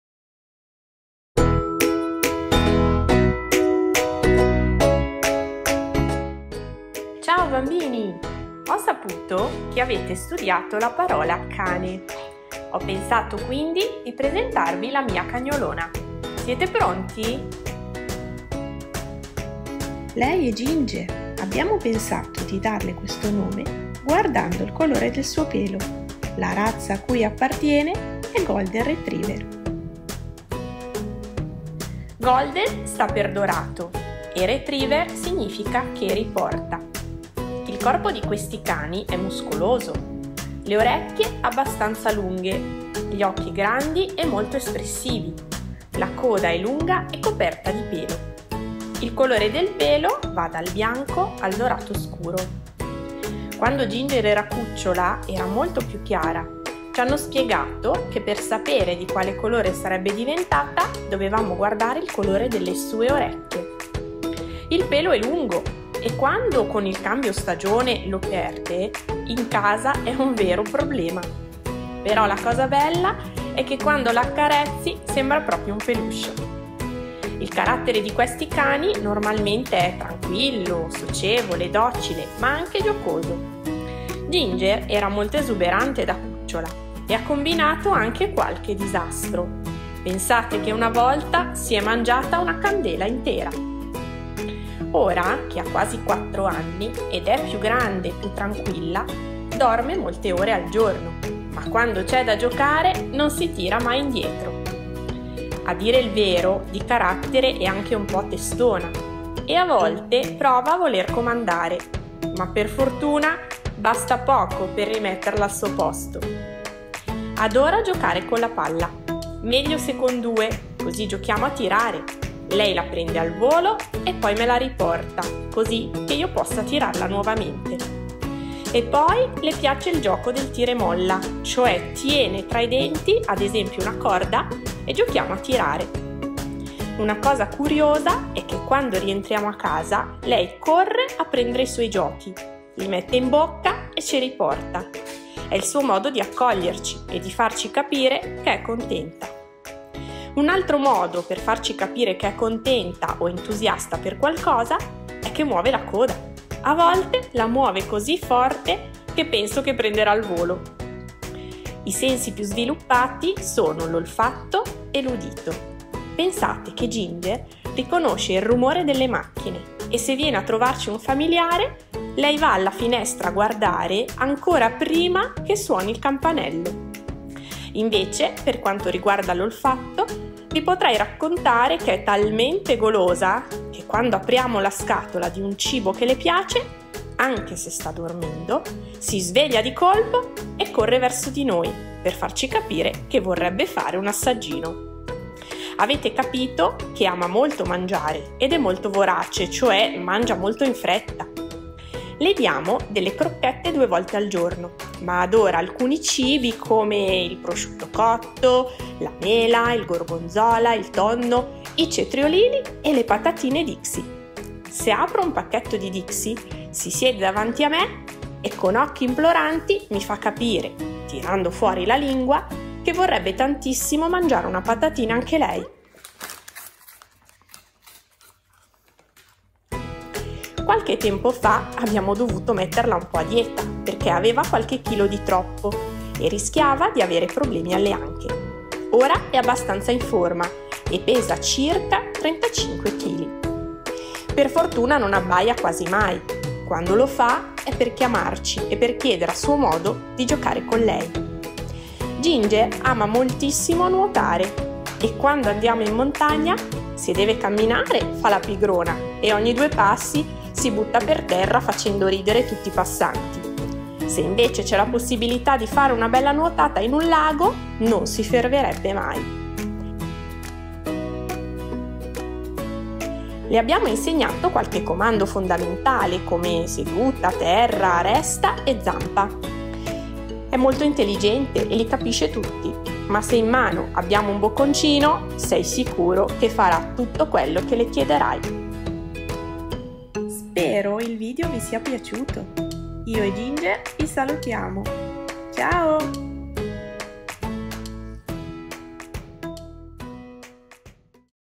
Vi presento Ginger! Raccontiamo la nostra golden retriever ai bimbi e ai grandi. :-)
Ecco il video che abbiamo preparato per i bimbi della scuola di Giovanna, che hanno studiato la parola "cane", per presentare loro un'esemplare della specie!